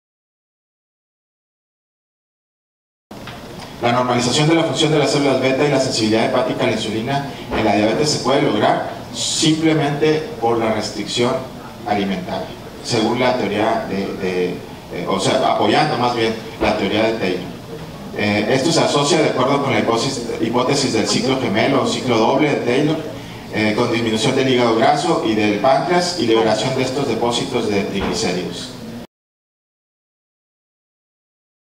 Dr Jesús Arguelles
La normalización de la función de células beta

Mecanismos de control de la DM tipo II con Cirugía
I Congreso Nacional del Manejo Multidisciplinario y Quirúrgico para Diabetes Tipo 2